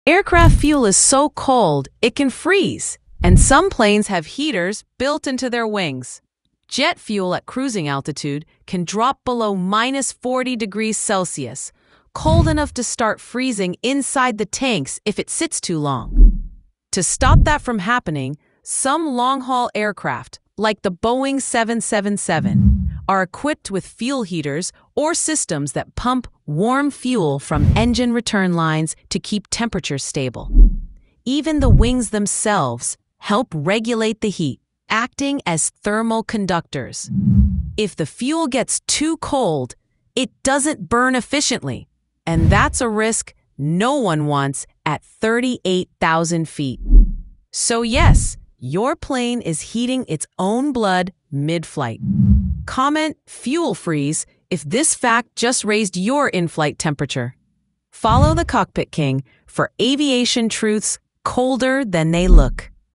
Keeping Jet Fuel Warm at 38,000 Feet
At 35,000 feet, it’s so cold that jet fuel can literally start to freeze — but planes are ready for it.
Modern jets have fuel and wing heating systems to keep fuel flowing and engines burning smoothly, even in extreme cold.
No heat? No thrust. No flight.

Comment “COLD FUEL” if you never knew planes had to fight freezing at altitude.